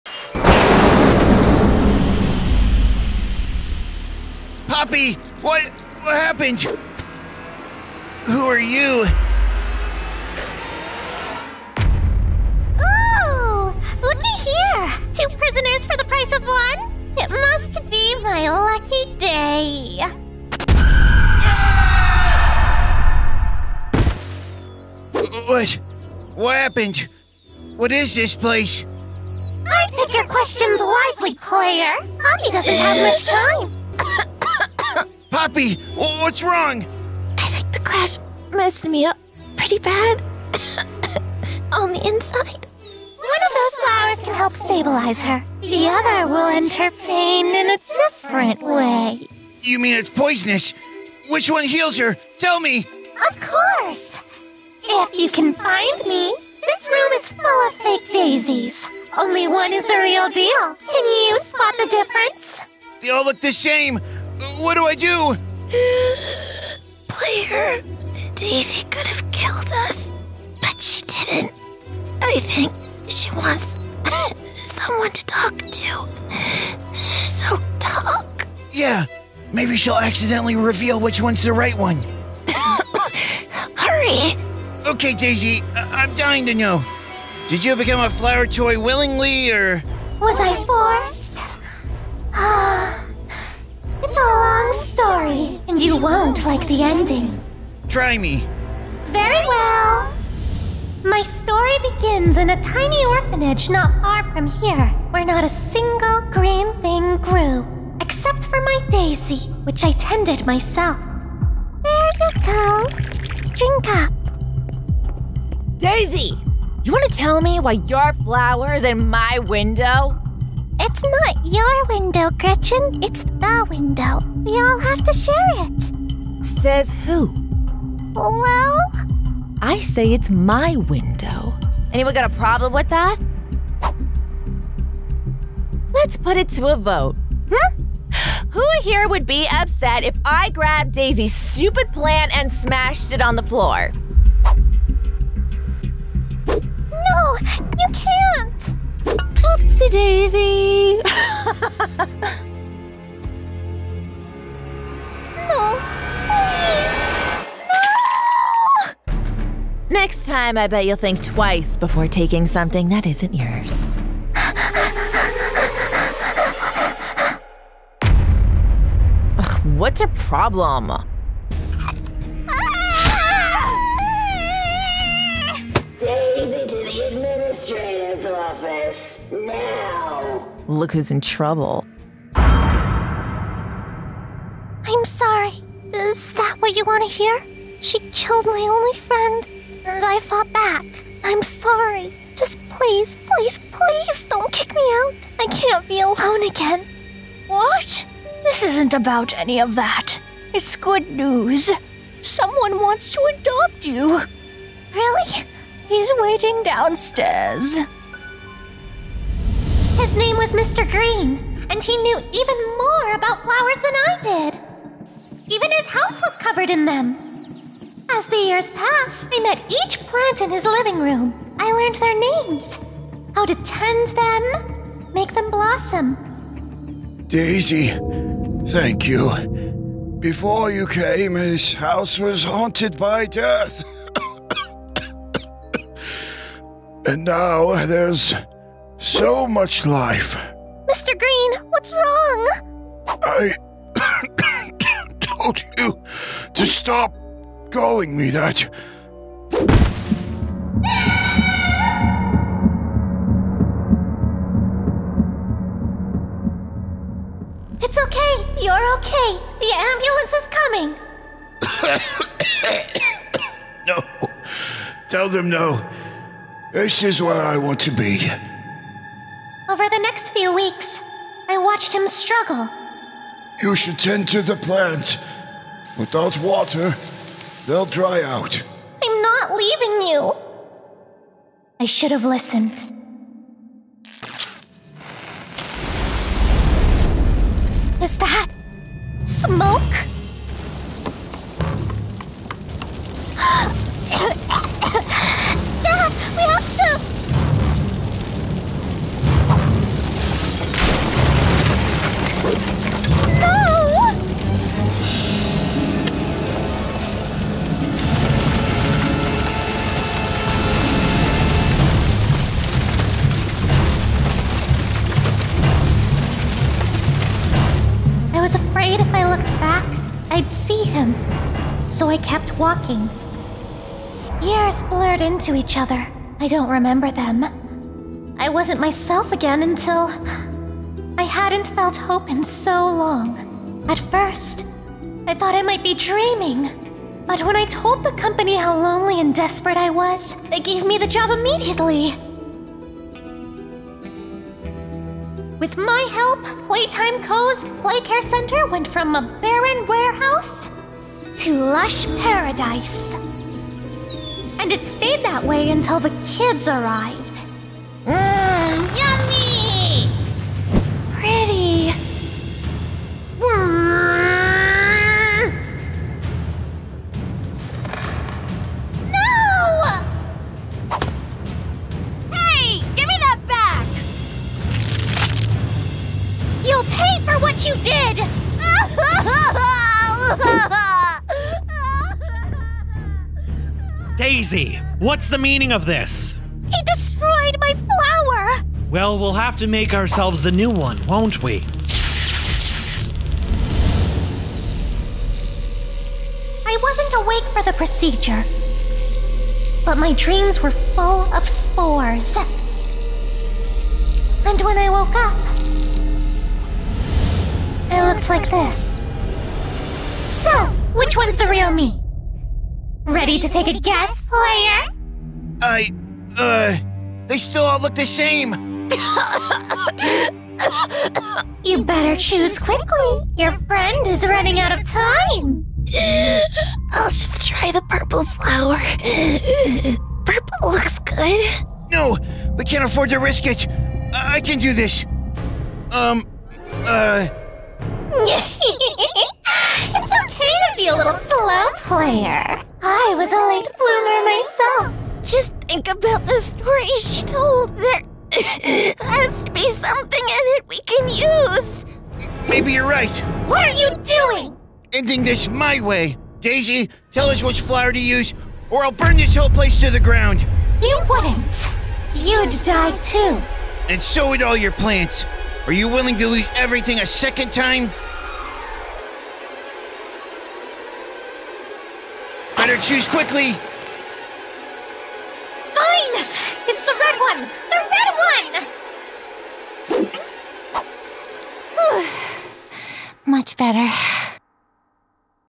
DAISY SAD ORIGIN STORY... (Cartoon Animation)
► Looking for a new PC? We’ve partnered with Apex Gaming with my own line of PCs!

Daisy, the evil flower monster, kidnaps Player and Poppy after a train crash. Now Player must find out Daisy's weakness before Poppy succumbs to her poisonous wound. Daisy reveals her sad back story and how she turned from little girl to horrible plant abomination.